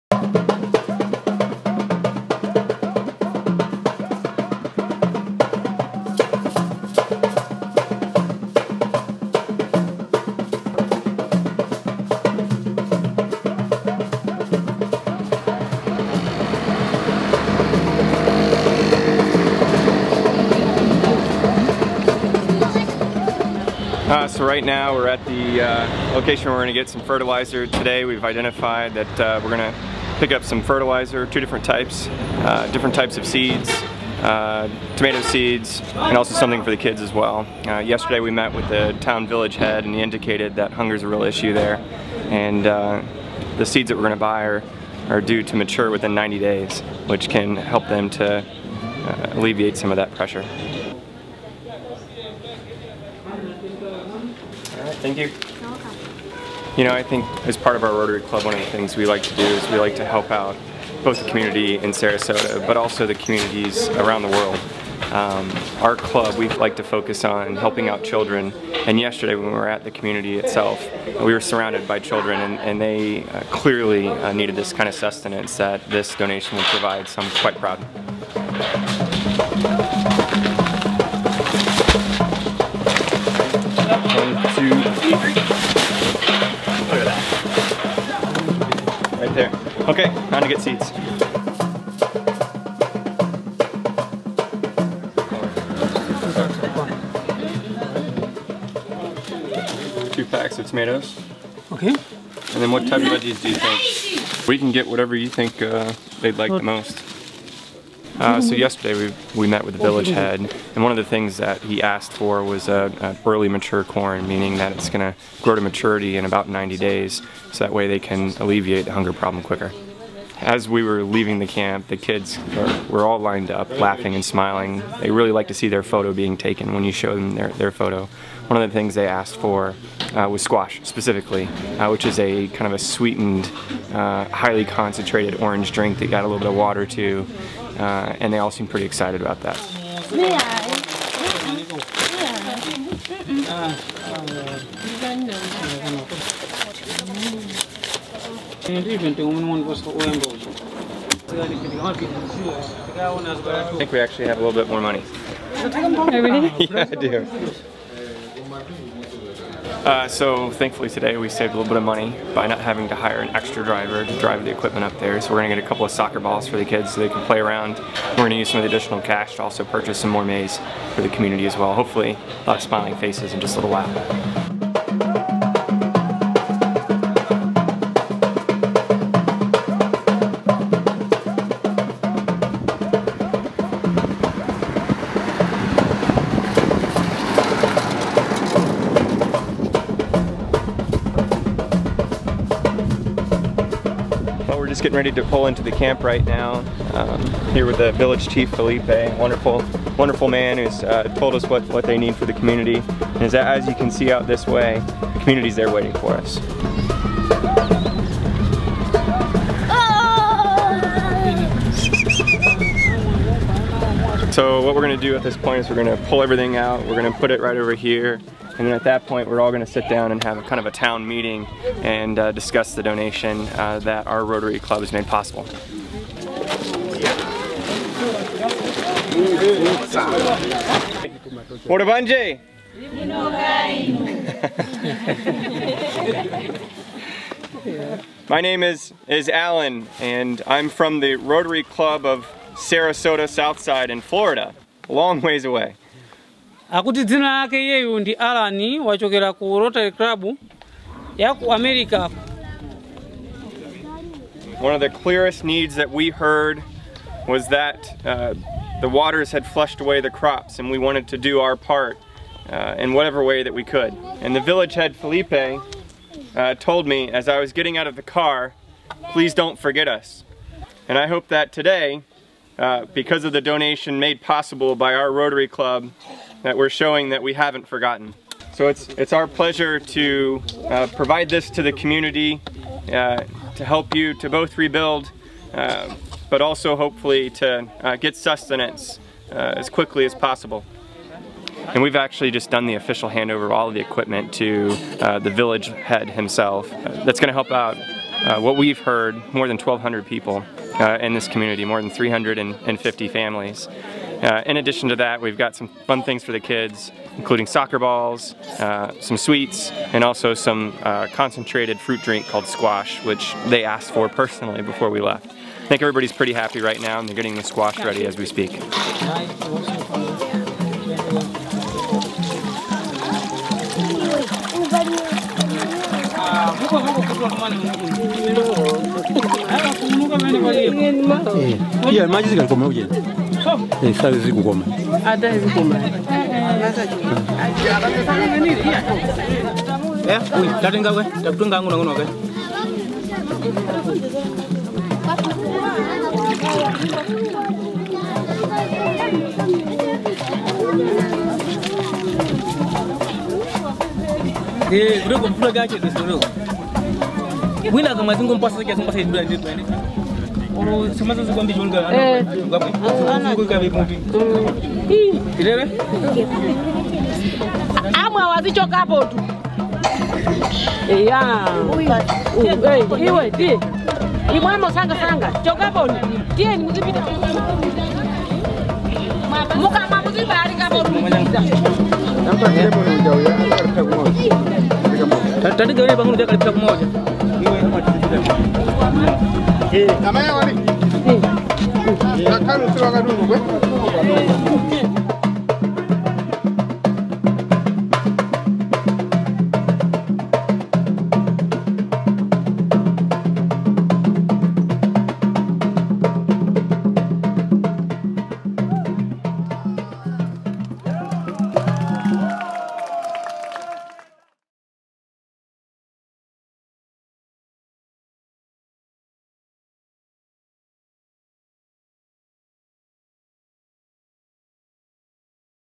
Rotary Club of Sarasota Southside Project in Malawi
The Rotary Club of Sarasota Southside saw a need in a small village in eastern Malawi.  Following flooding in early 2015, the Katumba village near the Mulanje mountain saw devastating floowing which washed away their crops.  The club visited community elders, listened to their needs, and came back with enough aid to assist more than a 1,000 villagers in their time of need.